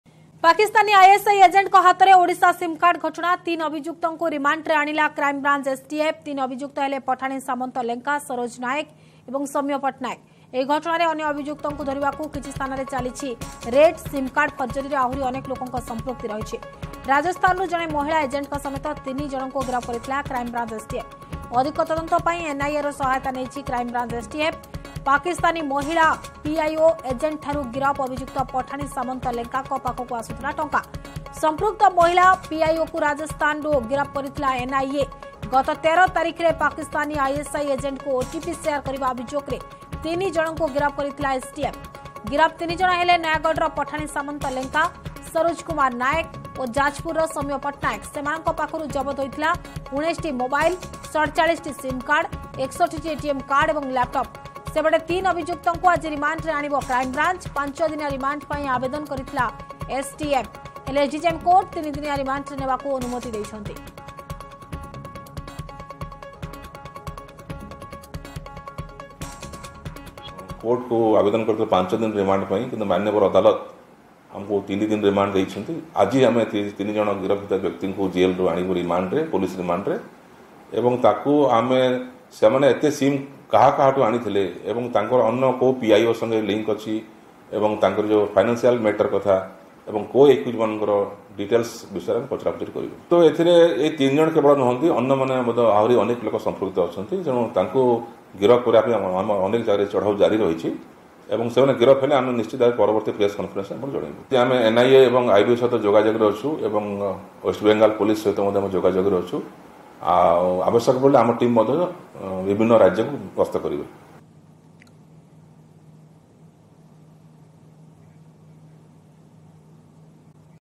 ମହିଳା ପିଆଇଓଙ୍କୁ ରାଜସ୍ତାନରୁ ଗିରଫ କରିଥିଲା NIA | Breaking News । Odia News
The Special Task Force (STF) of Odisha police has decided to invoke the National Security (NSA) Act against the mastermind who procured huge numbers of SIM cards and shared one-time passwords (OTP) with some criminals and anti-national elements including some Pakistani Intelligence Operatives (PIO) and ISI agents in Pakistan.
In course of investigation, the STF arrested Pathanisamant Lenka, who works as ITI Teacher and the mastermind of the case, Saroj Kumar Nayak, and Soumya Pattanaik and found out that the trio had links with the ISI agents living in Delhi, Rajasthan, Gujarat and West Bengal.
The STF is trying its best to know the source of purchasing huge numbers of SIM cards and since when they are in link with the PIO and ISI agents.